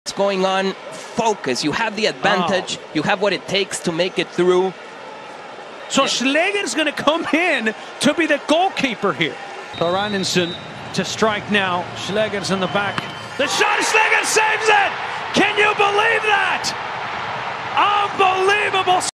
Increíble e histórico lo que hizo Rodrigo Schlegel en las eliminatorias de la MLS Cup
El Orlando City se jugaba el pase a la semifinal de la MLS Cup 2020 contra el NYC. Ya había comenzado la tanda de penaltis cuando una incomprensible decisión del árbitro envió al portero titular, el internacional peruano Pedro Gallese a los vestuarios. Una segunda amarilla por adelantarse antes de que el jugador de NYC tirase el penalti. Sin embargo ahí estaba el defensor argentino Rodrigo Schlegel que se puso los guantes y se convirtió en héroe al detener el penalti que dio el pase a las semifinales a los leones de Orlando.

El jugador español Uri Rosell jugó casi los 90 minutos de titular para Orlando City.